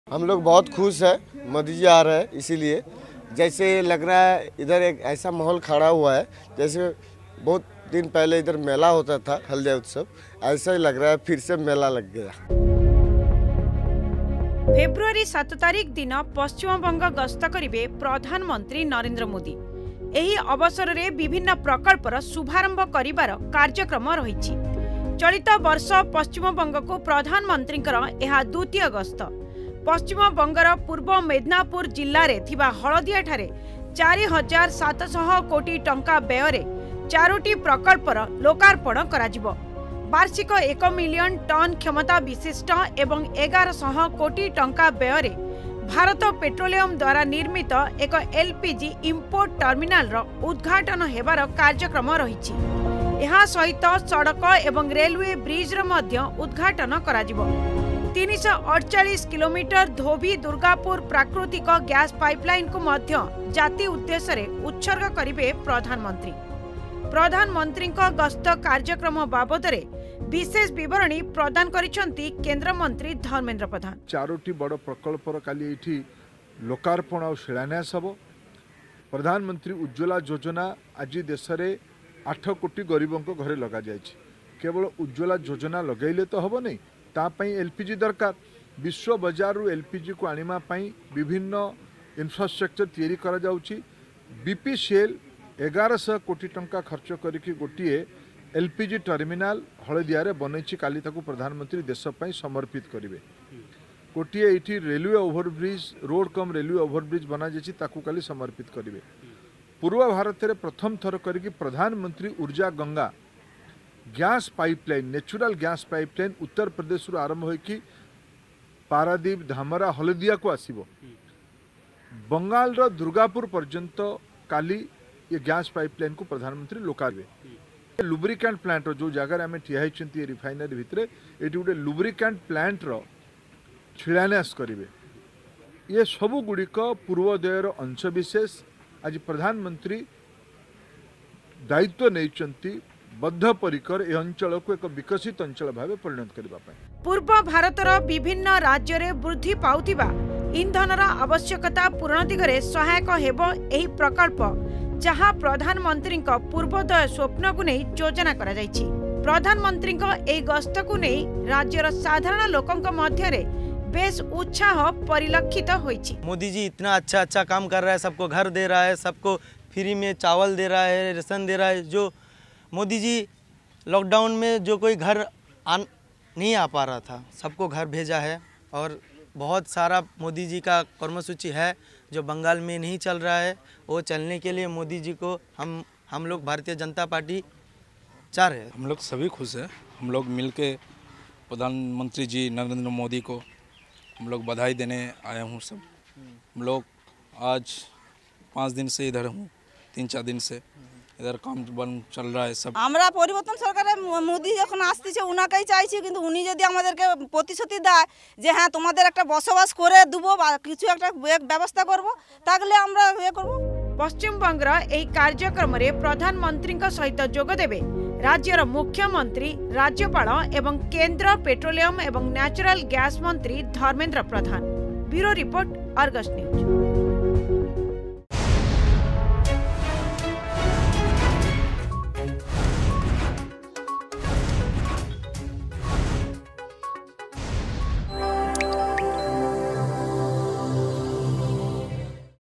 ଫେବୃଆରୀ ୭ ତାରିଖ ଦିନ ପଶ୍ଚିମବଙ୍ଗ ଗସ୍ତ କରିବେ ପ୍ରଧାନମନ୍ତ୍ରୀ ନରେନ୍ଦ୍ର ମନ୍ତ୍ରୀ
ଫେବୃଆରୀ ୭ ତାରିଖ ଦିନ ପଶ୍ଚିମବଙ୍ଗ ଗସ୍ତ କରିବେ ପ୍ରଧାନମନ୍ତ୍ରୀ ନରେନ୍ଦ୍ର ମନ୍ତ୍ରୀ । ଏହି ଅବସରରେ ବିଭିନ୍ନ ପ୍ରକଳ୍ପର ଶୁଭାରମ୍ଭ କରିବାର କାର୍ଯ୍ୟକ୍ରମ ରହିଛି । ଚଳିତ ବର୍ଷ ପଶ୍ଚିମବଙ୍ଗକୁ ପ୍ରଧାନମନ୍ତ୍ରୀଙ୍କର ଏହା ଦ୍ୱିତୀୟ ଗସ୍ତ । ଏହି କାର୍ଯ୍ୟକ୍ରମକୁ ନେଇ ରାଜ୍ୟର ଲୋକମାନଙ୍କ ମଧ୍ୟରେ ବେଶ୍ ଉତ୍ସାହ ପରିଲକ୍ଷିତ ହୋଇଛି । ପଶ୍ଚିମବଙ୍ଗର ଏହି କାର୍ଯ୍ୟକ୍ରମରେ ତାଙ୍କ ସହିତ ଯୋଗଦେବେ ରାଜ୍ୟର ମୁଖ୍ୟମନ୍ତ୍ରୀ, ରାଜ୍ୟପାଳ ଏବଂ କେନ୍ଦ୍ର ପେଟ୍ରୋଲିୟମ ଏବଂ ନ୍ୟାଚୁରାଲ ଗ୍ୟାସ୍ ମନ୍ତ୍ରୀ ଧର୍ମେନ୍ଦ୍ର ପ୍ରଧାନ ...